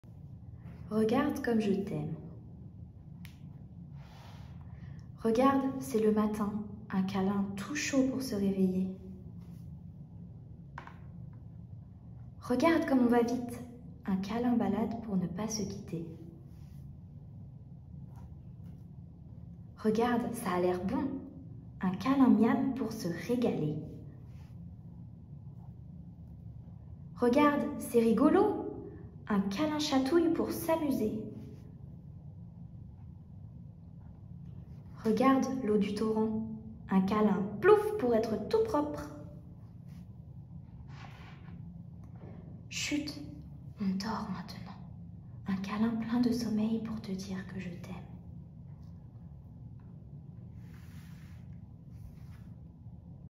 Regarde comme je t'aime 🐻 | Lecture livre pour enfants
📚 Lecture de "Regarde comme je t'aime".
Le bébé retrouve dans cet album l'expérience d'une relation tendre et chaleureuse qu'il connaît bien. Ce lien participe à la construction de sa sécurité affective qui lui permettra d'être actif et d'explorer le monde.

👉 Profitez de la livraison gratuite dans le monde (0,01€ en France métropolitaine)

Envie de nous partager votre avis sur cet album jeunesse ? Alors laissez-nous un commentaire !